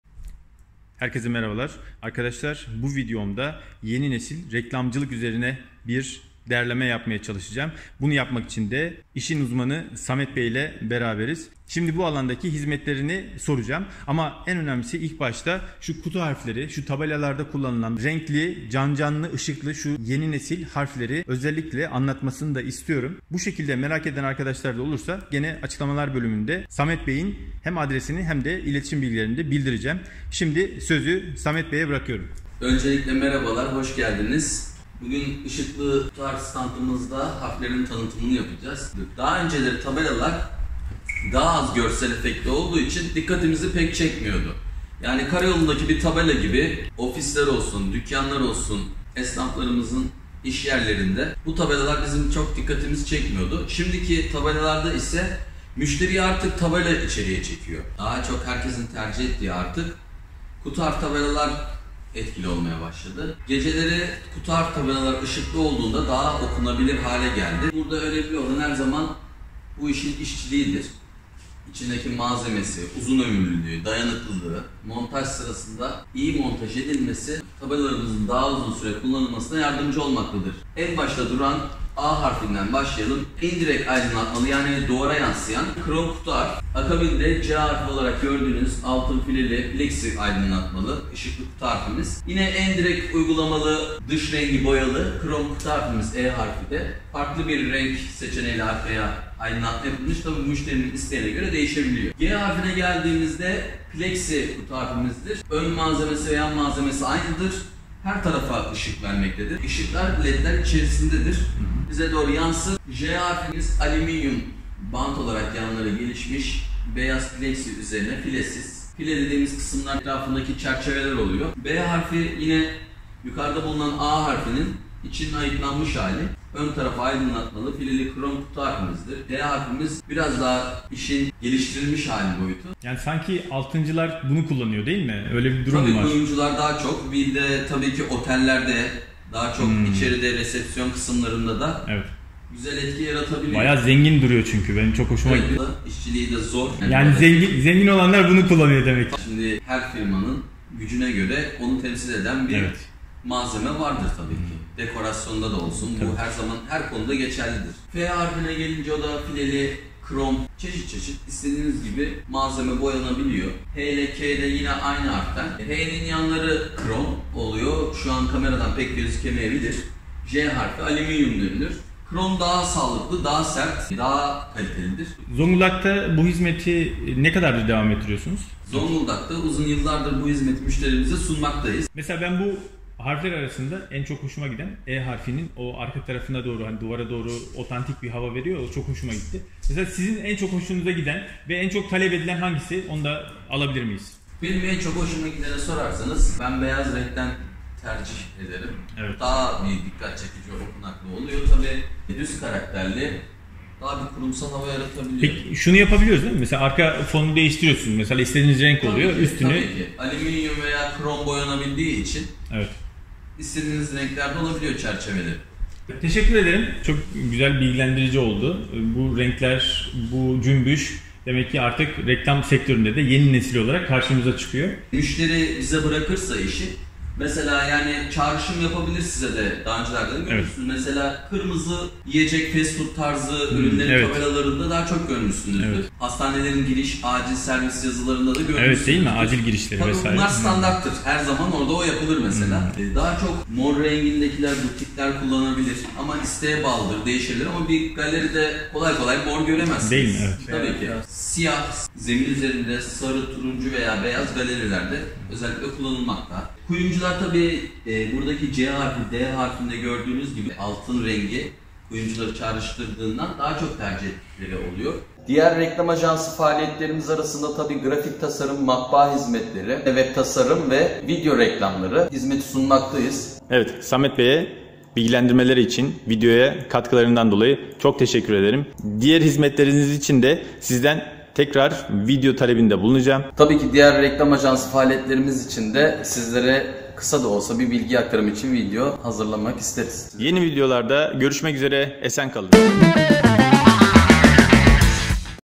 Yeni Nesil 3 Boyutlu Reklam Tabelası Nasıl Olur? İşin Ustasına Sorduk!
İşin püf noktası. En ideal reklam tabelası seçimi nasıl olmalıdır? gibi soruların cevabını size veren bir video derlemeye çalıştım.
Bu konuda Zongudak'ta hizmet veren işini en iyi yapan firmalardan biri olan Adlgrafik reklam ajansı sahibi Samet Bey bilgilerini bizimle paylaştı.

KANALIMDA SİZE GÖRE BİR ÇOK VİDEO VAR. BİR GÖZATIN
Facebook sayfamızda da sizi bekliyoruz.

YAP İŞLET SEYRET